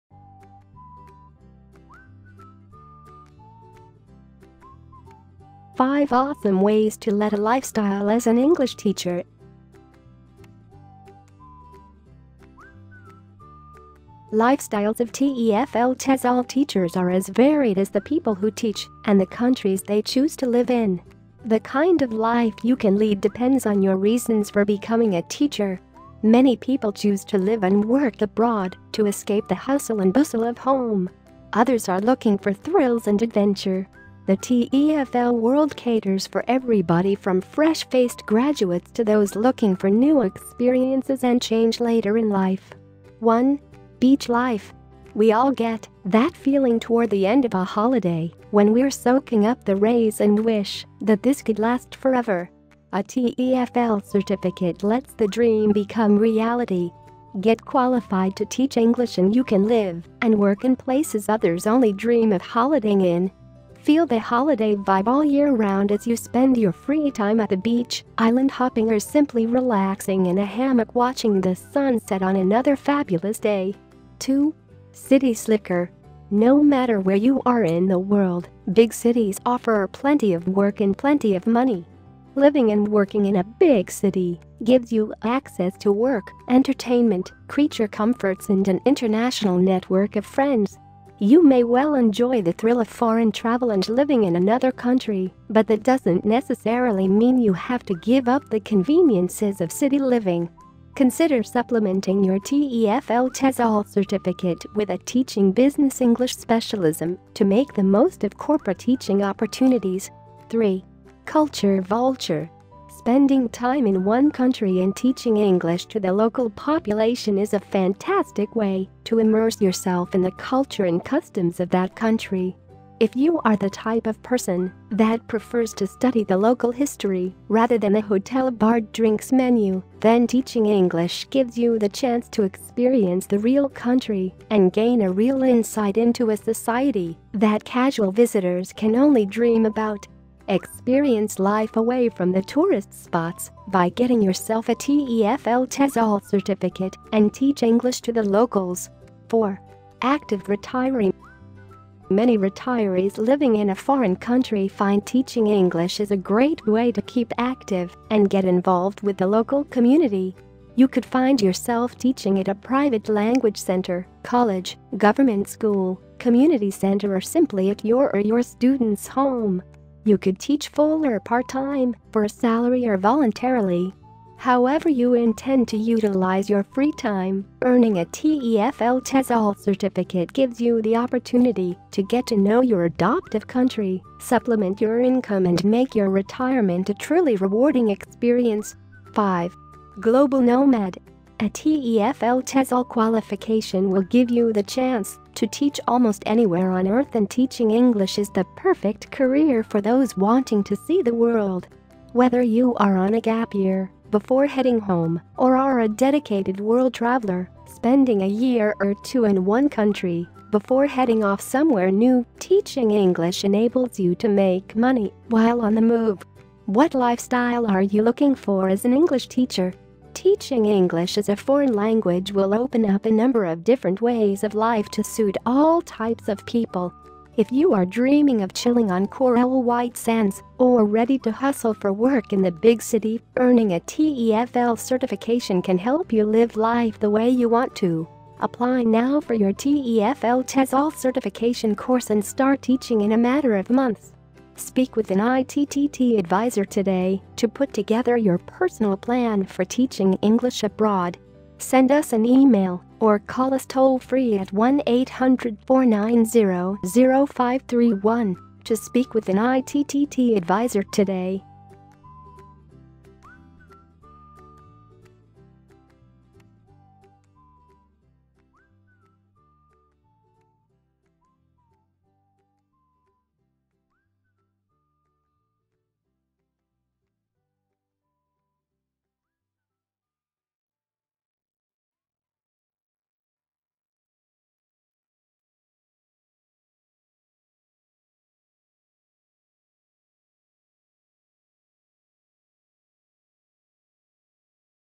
5 Awesome Ways to Lead a Lifestyle as an English Teacher | ITTT TEFL BLOG
Lifestyles of TEFL/TESOL teachers are as varied as the people who teach and the countries they choose to live in. The kind of life you can lead depends on your reasons for becoming a teacher. Many people choose to live and work abroad to escape the hustle and bustle of home. Others are looking for thrills and adventure. The TEFL world caters for everybody from fresh faced graduates to those looking for new experiences and change later in life.